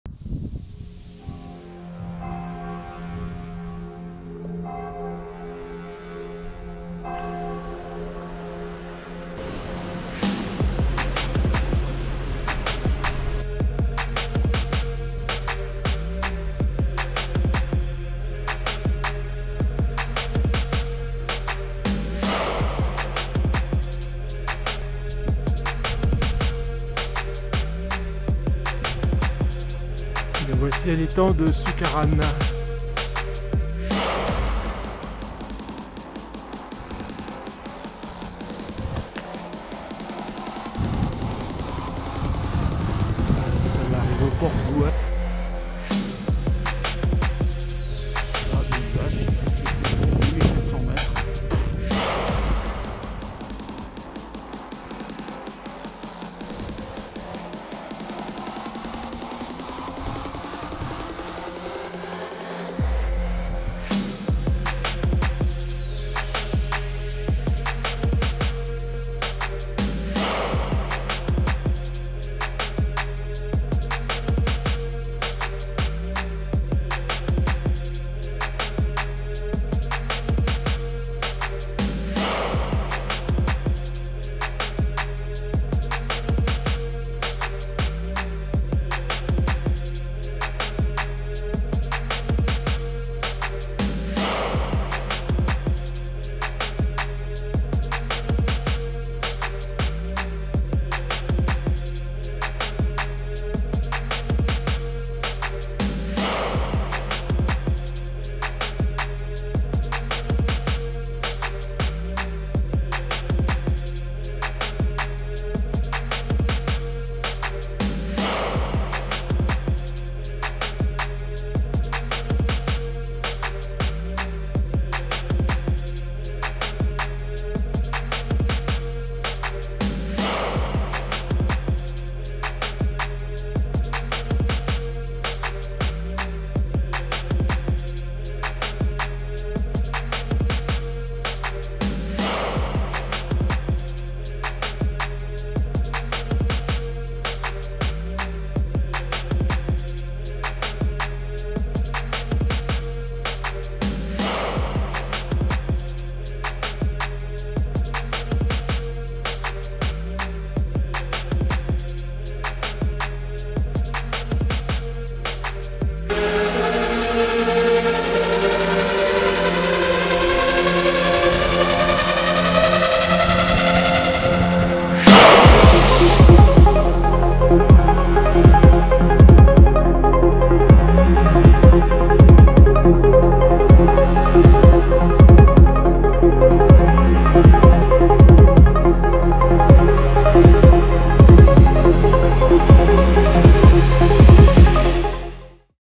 Pic de la Soucaranne
Boucle, départ barrage de Soulcem, étang de la Soucaranne, port Bouet, pic de la Soucaranne ou pic de la Rouge, col Roumazet, étang de Roumazet.